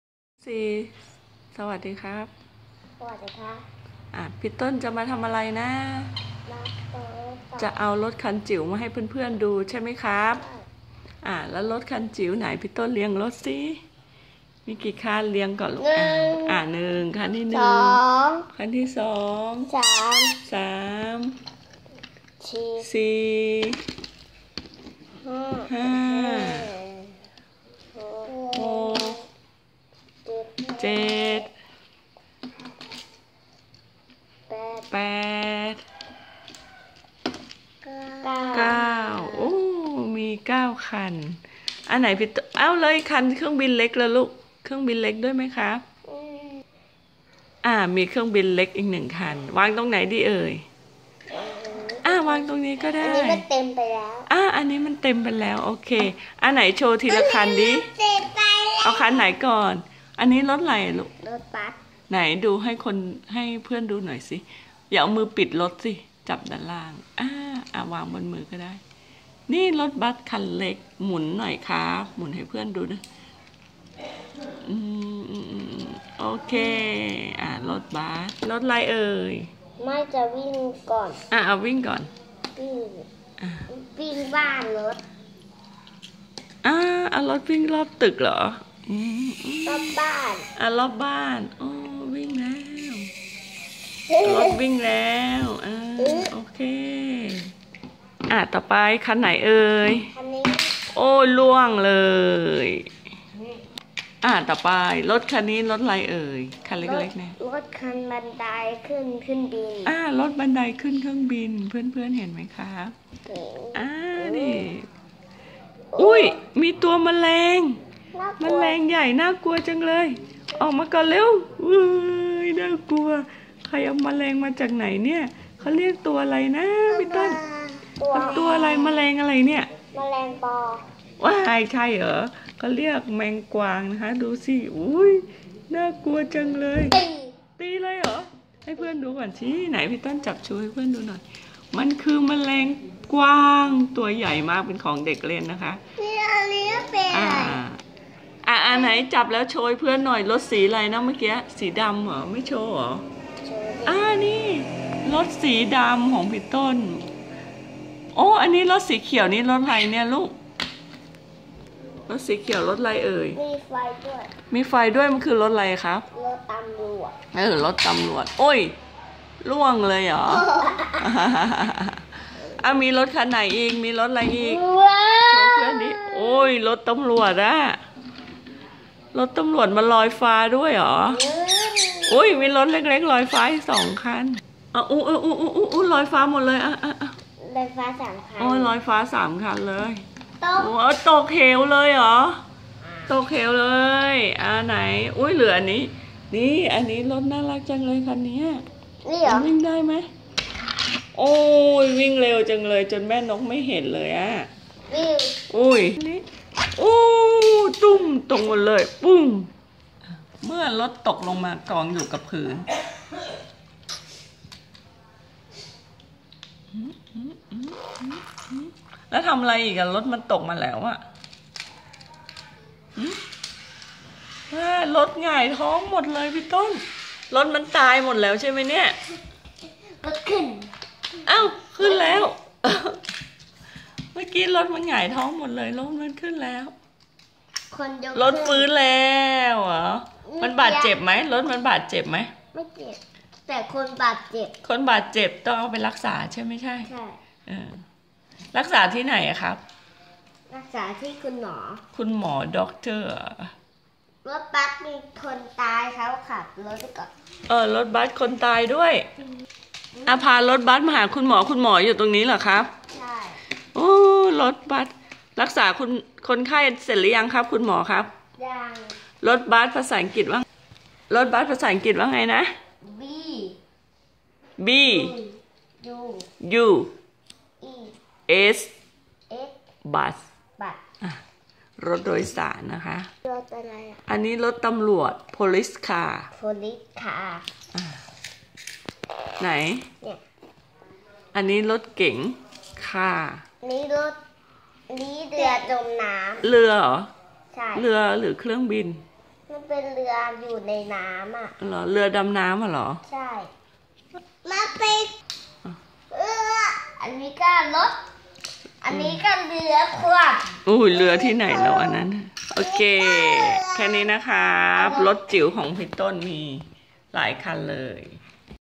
รถคันจิ๋ว/พี่ต้นไม้
พี่ต้นเอารถคันจิ๋วมาให้เพื่อนได้ชมกันว่ามีกี่คัน อะไรบ้าง มาดูกันครับ